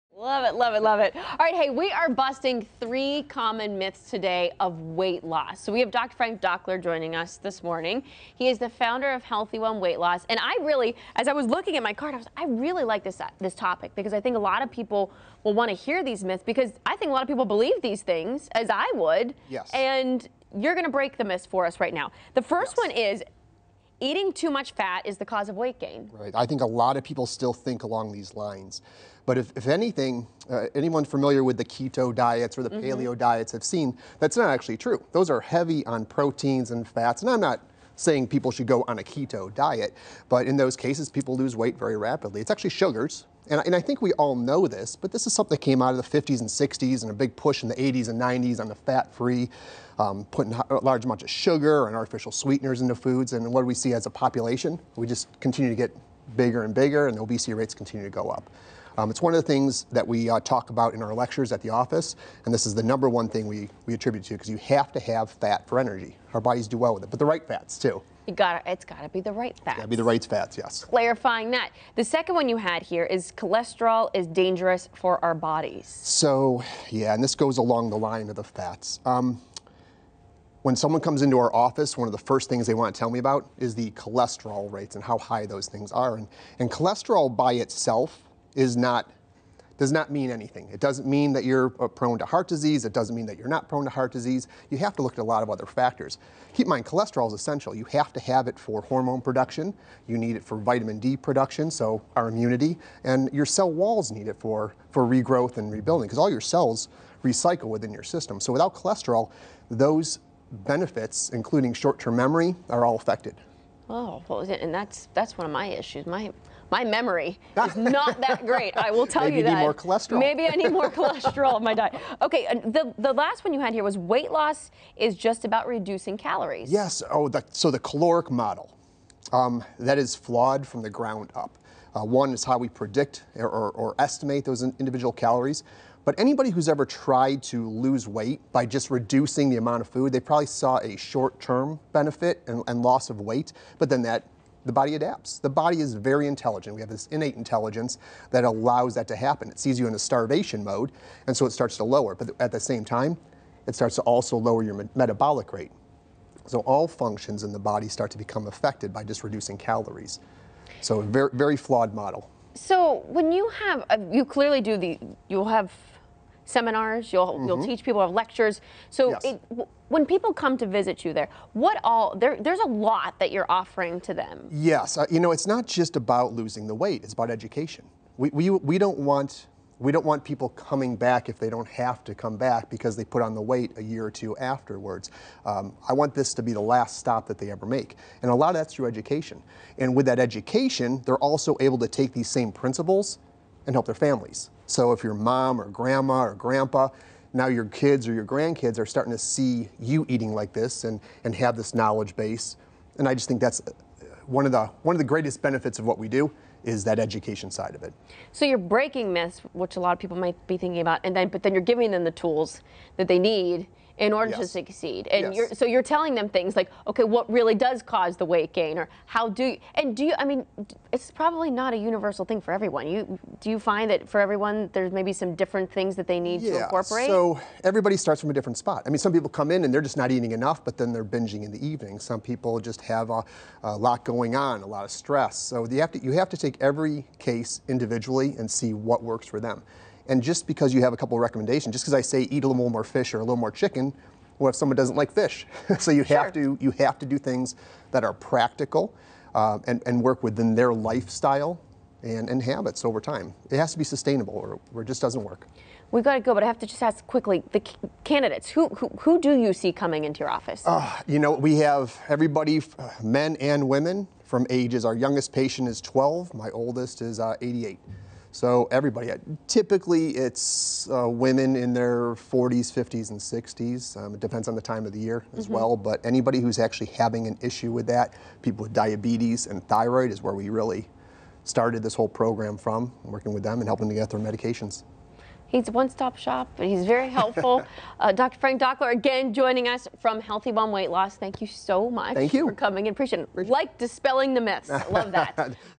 3 Diet Myths
There is a lot of bad information out there and so many people that are struggling to lose that extra body fat.  Here are three (3) Diet Myths that you may NOT know